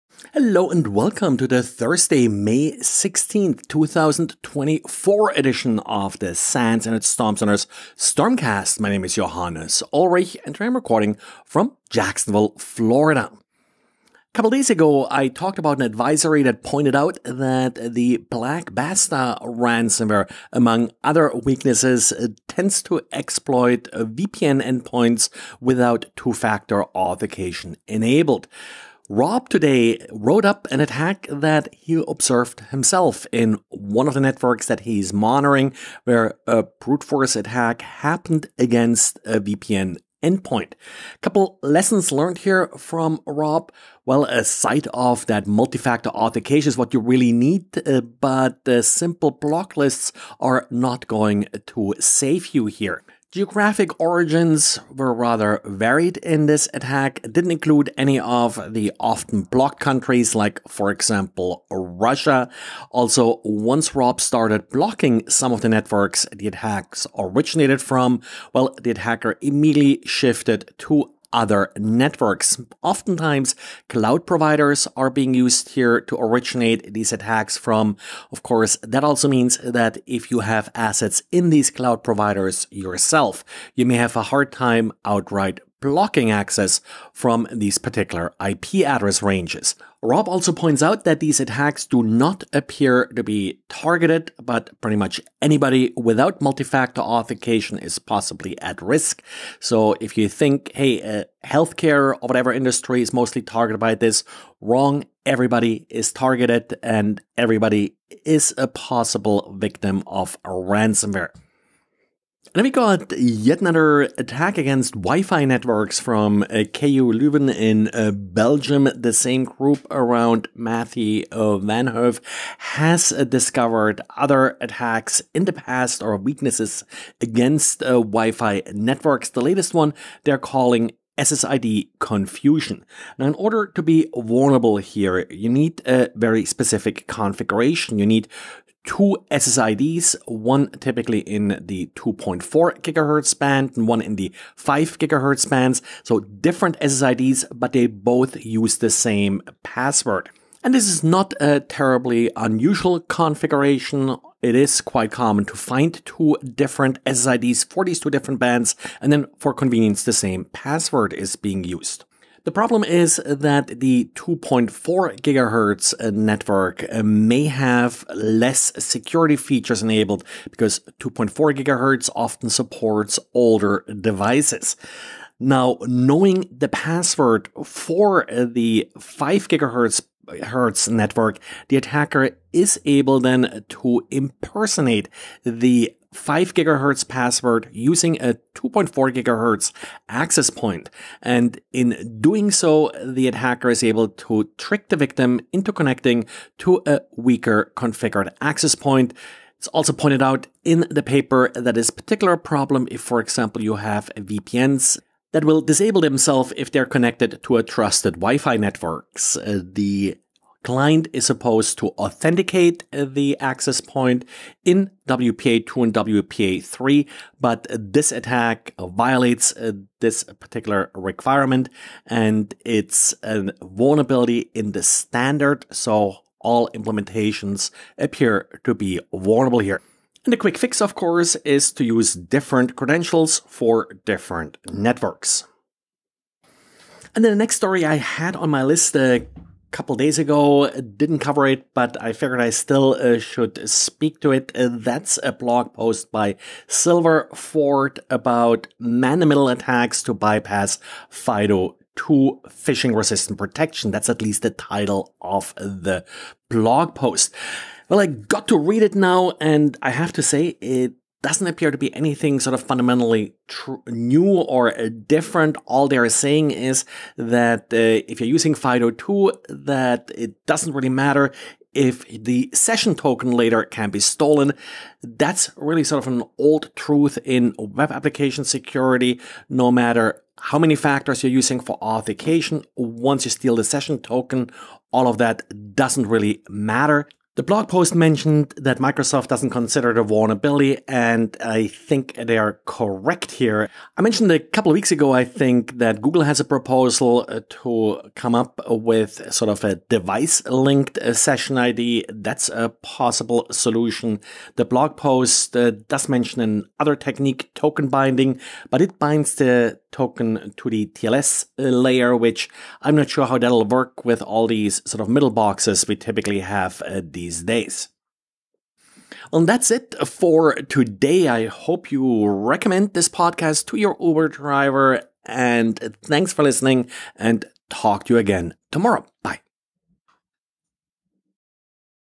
Network Security News Summary for Thursday May 16th, 2024
VPNs need MFA; SSID Confusion; FIDO2 Session Hijacking

Got MFA? If not, now is the time!

SSID Confusion: Making Wi-Fi Clients Connect to the Wrong Network CVE-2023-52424